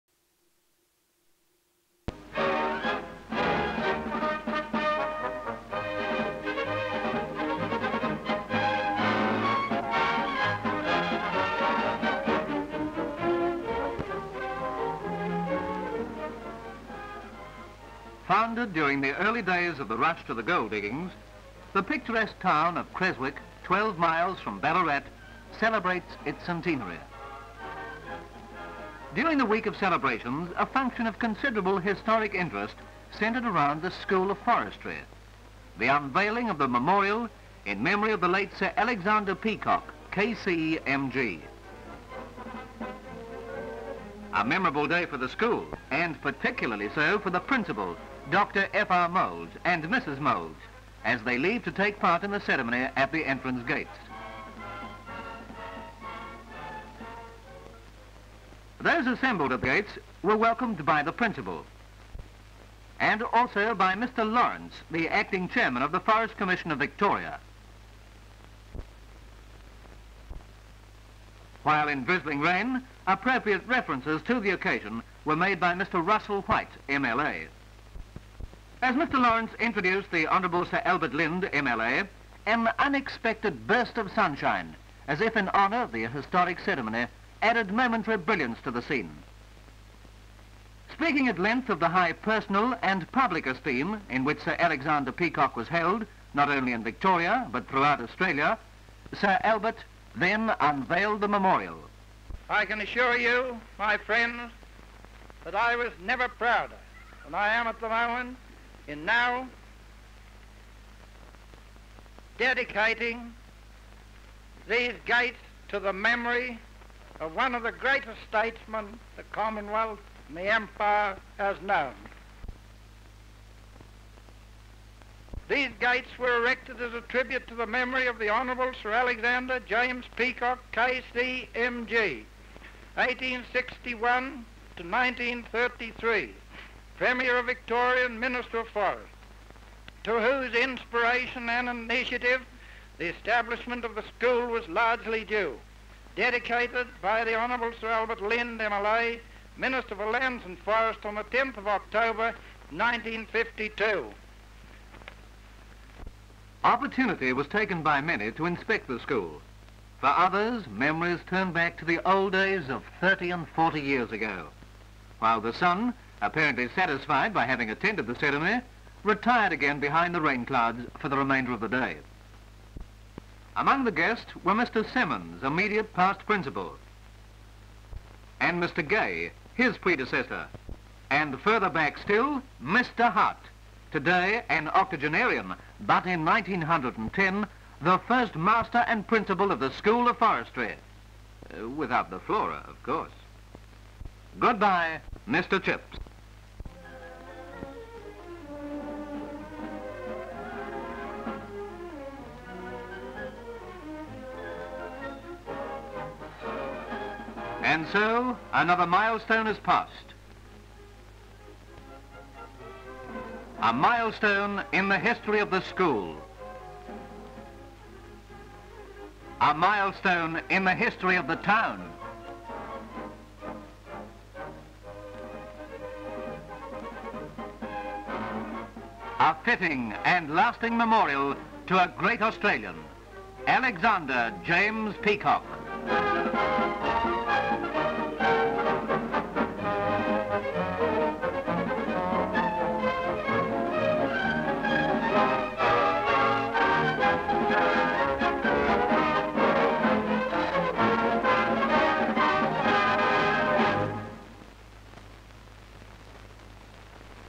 Dedication of the 'Peacock' Memorial Gates - Victorian School of Forestry, Creswick. 1952
Australia's first Forestry School opened in Creswick, in Central-Victoria in 1910. This film captures a dedication of memorial gates at the Victorian School of Forestry (VSF) in1952.
Alexandra Peacock was born in Creswick in 1861, and went on to represent the area in State Parliament for 44 years, serving as Premier on three occasions.
The film has local historical  significance; and several VSF Principals are featured.
Produced: 'Cine News - for the Victorian Forests Service'. (+ sound).